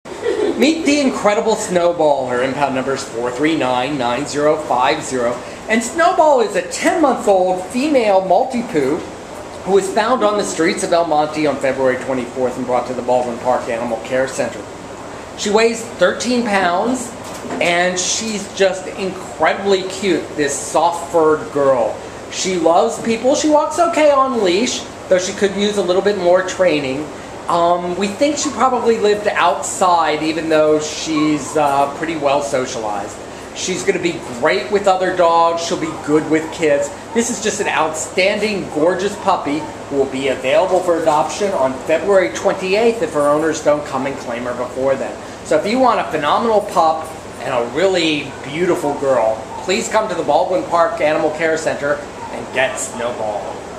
Snowball A4399050 ADOPTED!!!
Snowball is a mellow, low energy ten month old white female Maltipoo puppy who was found in El Monte on February 24th and brought to the Baldwin Park Animal Care Center.  Weighing thirteen pounds, Snowball walks well on leash and is well socialized.  She gets along with other dogs.  Snowball is an incredibly soft, silky puppy who likes to be held and cuddled and is happiest in a human lap.  Snowball will be the perfect indoor pet for anyone in any living situation.